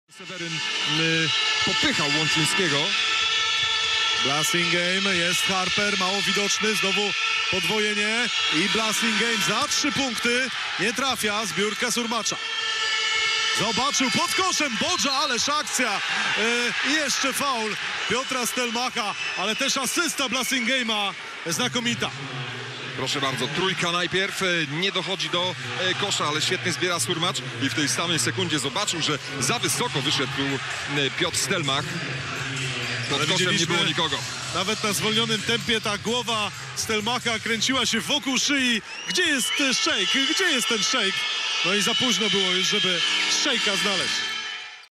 Świetne podanie Jerela Blassingame'a i akcja 2+ Cheikha Mbodja
zdjęcia: IPLA.tv
montaż: PLK